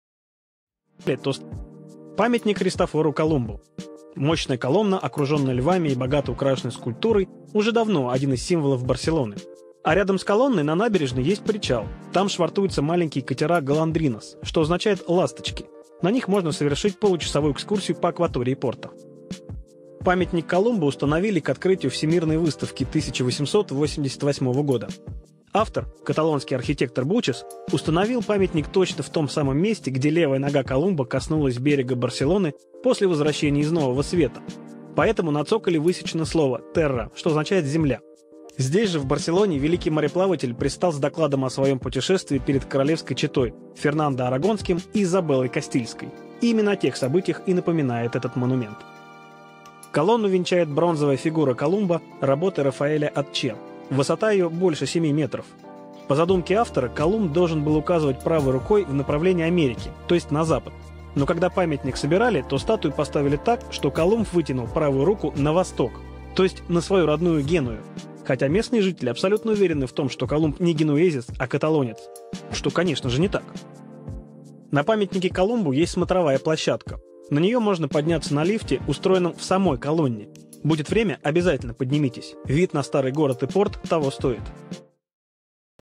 Памятник Христофору Колумбу | Достопримечательности Барселоны
Памятник Христофору Колумбу – мощная колонна, окруженная львами и богато украшенная скульптурой, – уже давно один из символов Барселоны. А совсем недалеко от колонны, на набережной есть причал. Там швартуются маленькие катера-голондринас (golondrinas), что означает «ласточки». На них можно совершить 30-минутную экскурсию по акватории порта.

Памятник Колумбу высотой в 50 метров установили к открытию Всемирной выставки 1888 года. Автор – каталонский архитектор Гаэта Бучас (Gaietà Buigas) установил ее точно в том самом месте, где левая нога Колумба коснулась берега Барселоны после возвращения из Нового Света. Поэтому на цоколе и высечено слово «Терра» (Tierra), что означает «земля». Здесь же в Барселоне великий мореплаватель предстал с докладом о своем путешествии перед королевской четой – Фердинандом Арагонским и Изабеллой Кастильской. Именно о тех событиях и напоминает монумент.

Колонну венчает бронзовая фигура Колумба работы Рафаэля Аче (Rafael Atché). Высота ее больше 7 м. По задумке авторов, Колумб должен был указывать правой рукой в направлении Америки, то есть на запад. Но когда памятник собирали, то статую поставили так, что Колумб вытянул правую руку на восток. То есть на свою родную Геную. Хотя местные жители абсолютно уверены в том, что Колумб – не генуэзец, а каталонец, что, конечно, не так.

На памятнике Колумбу есть смотровая площадка. На нее можно подняться на лифте, устроенном в самой колонне. Будет время, поднимитесь. Вид на Старый город и порт того стоит.